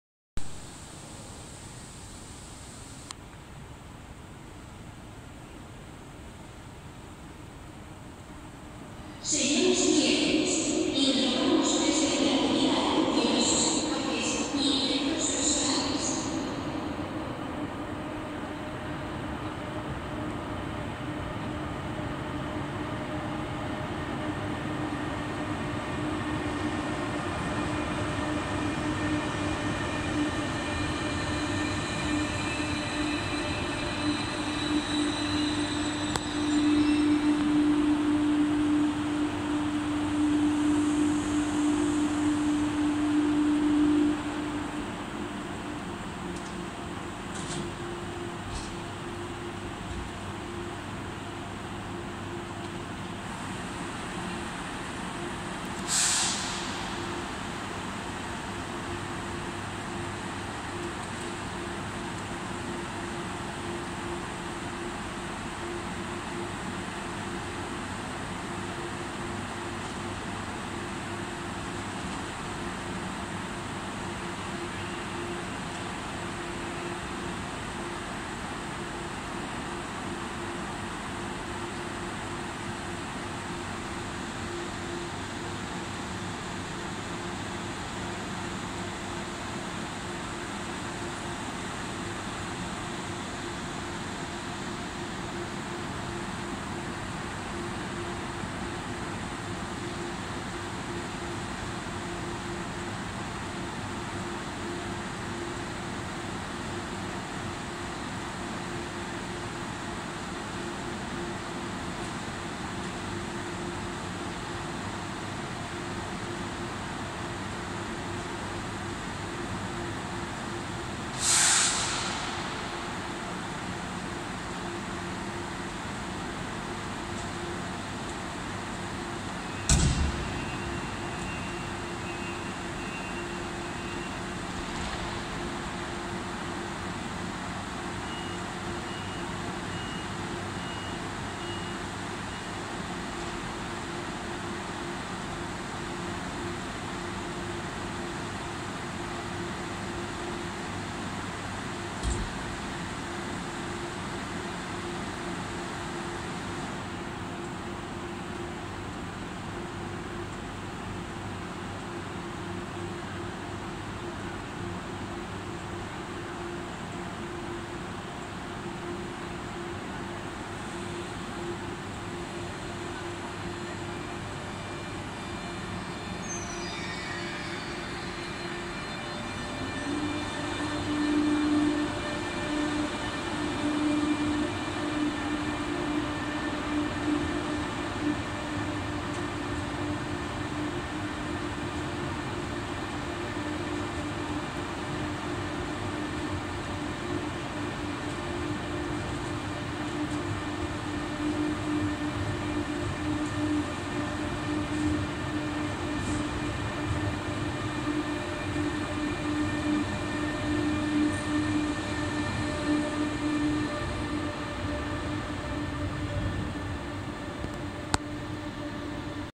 AVLO de Renfe entrando y saliendo de Zaragoza Delicias
Tren Madrid Puerta de Atocha a Barcelona Sants y Figueres Vilafant entrando y saliendo de la zaragozana estación de Delicias la tarde del jueves 14 de julio de 2021.